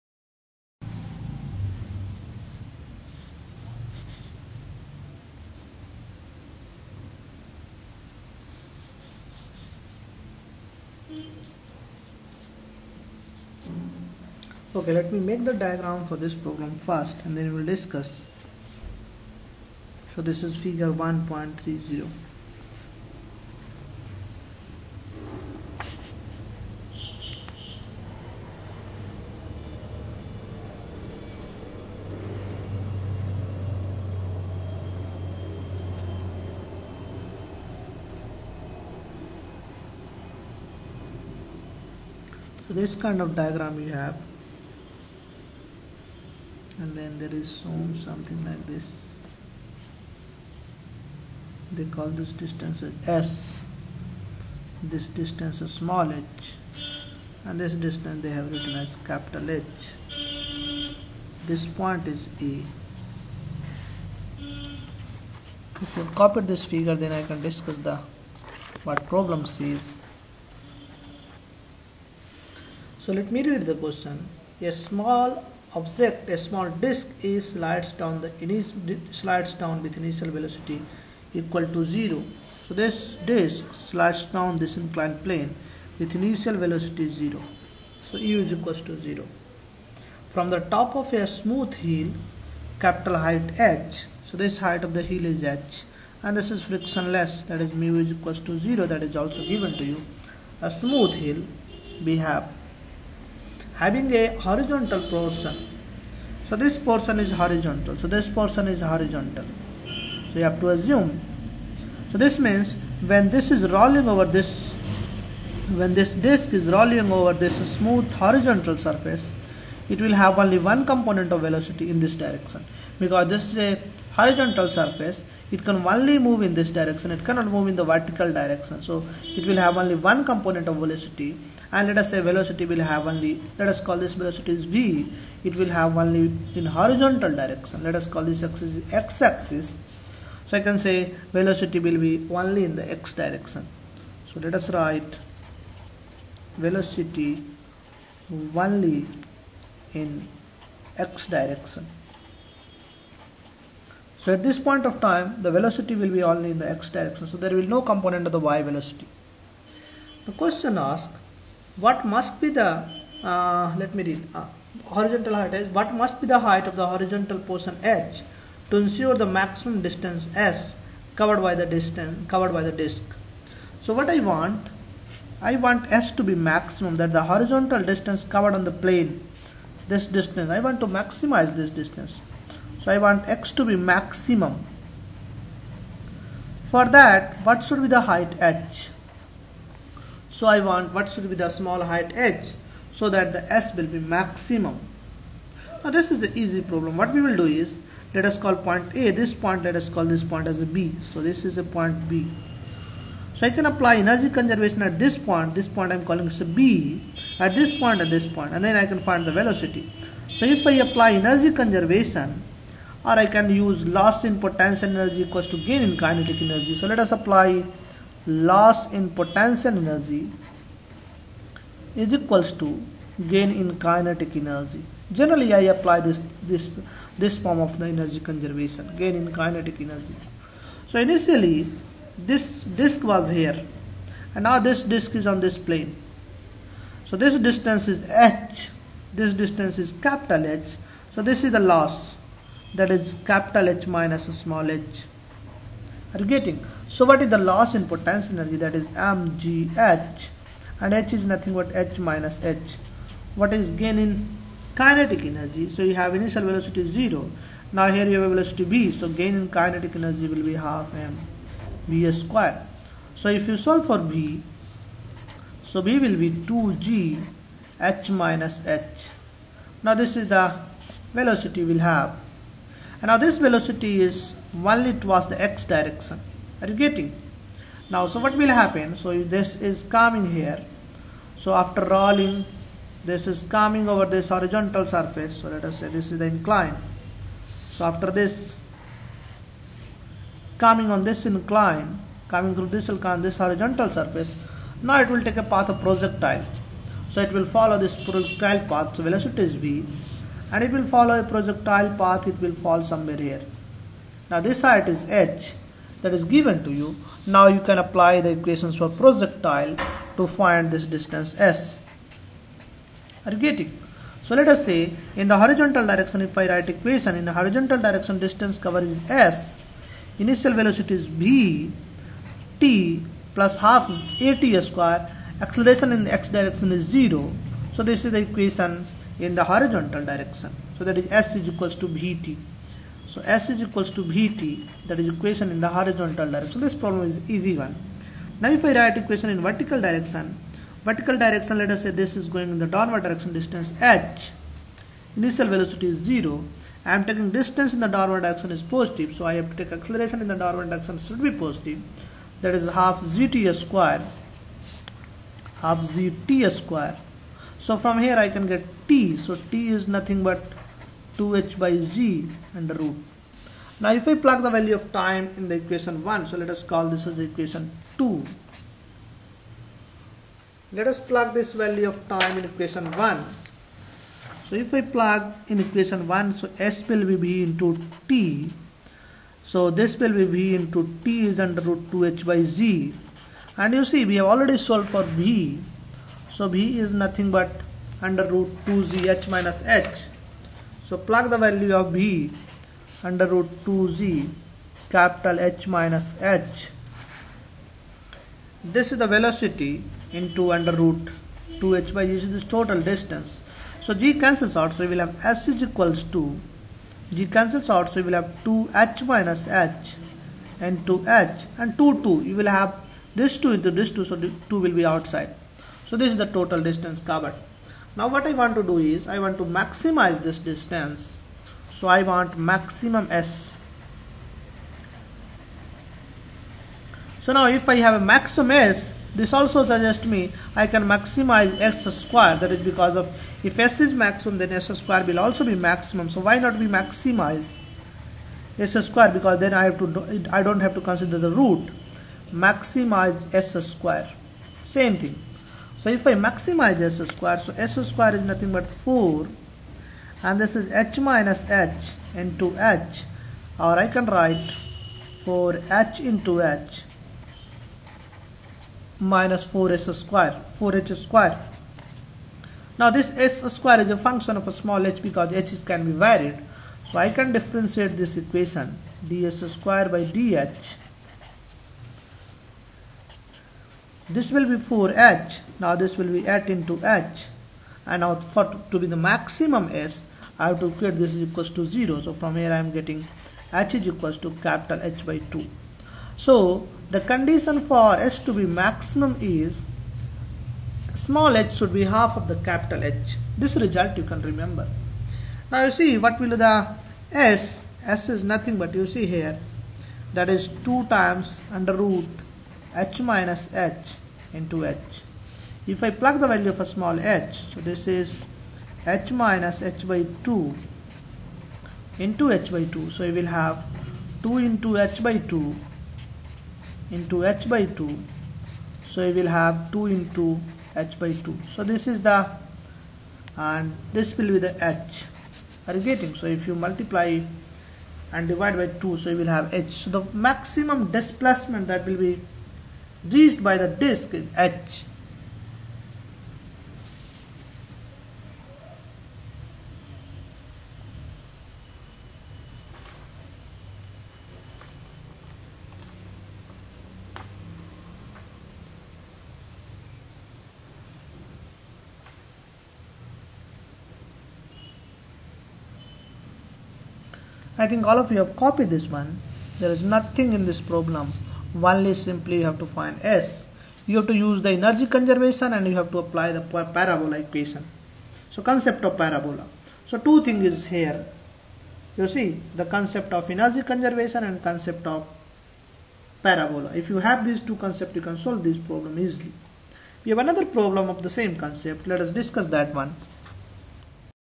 I.E. Irodov 1.135 I Work and Energy- Problem I Maximum distance by disc
Application of Work Energy conservation